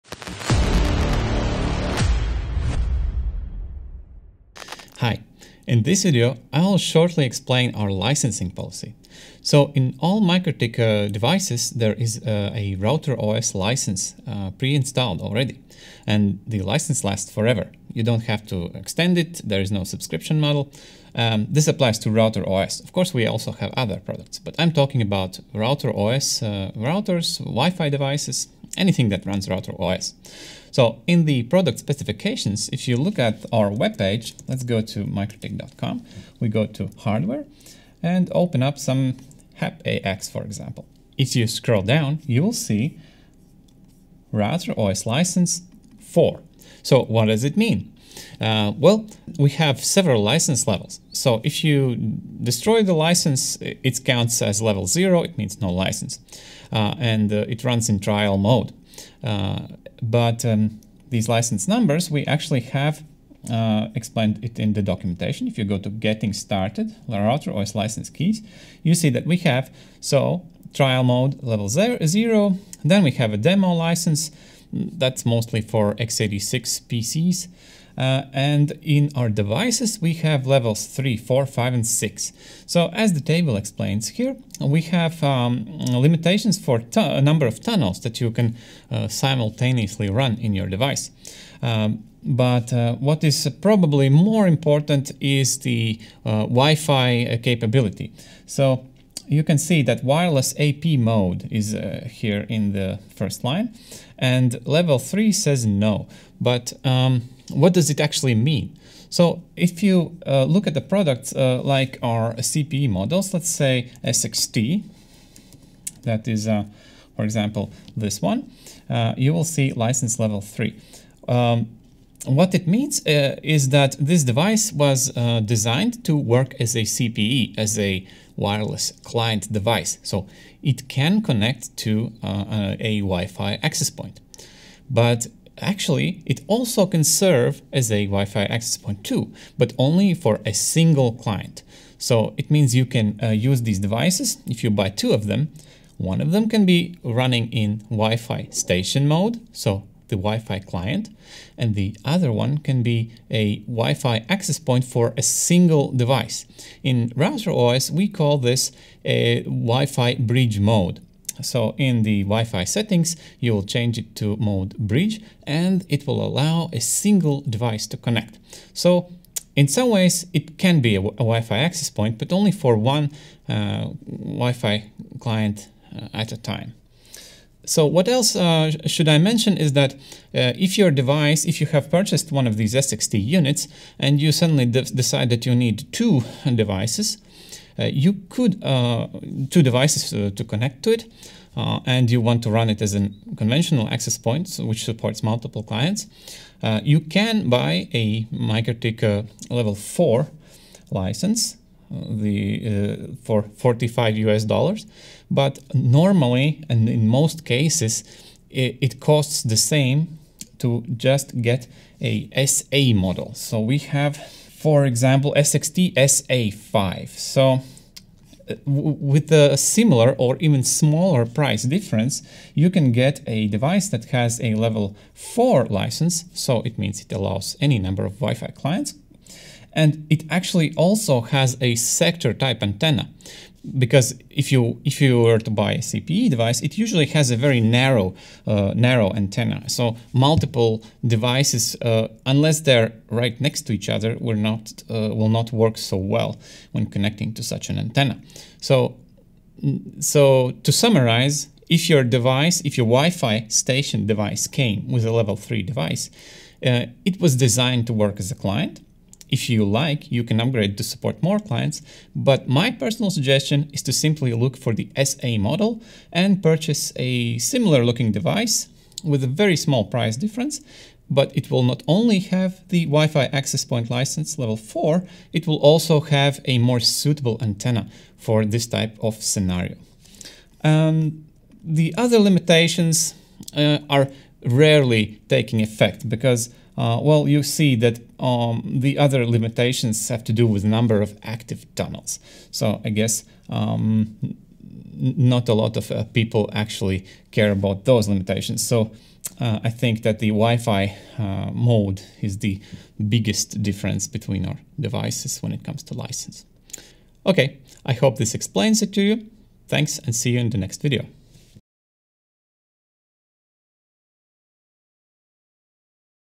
MikroTik device license types
This video explains the difference between MikroTik wireless device license levels. Difference between Level3 and Level4 license? Watch this video. Here is the relevant manual